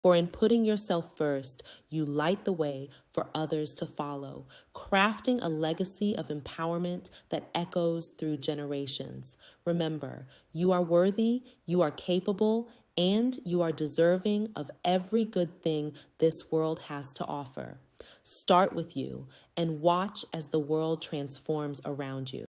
Crafting Your Legacy: Empowerment and Transformation in the Modern World
Discover the power of putting yourself first and creating a lasting impact through empowerment. Join us as we explore how prioritizing your worth and capabilities can transform not only your life but also the world around you.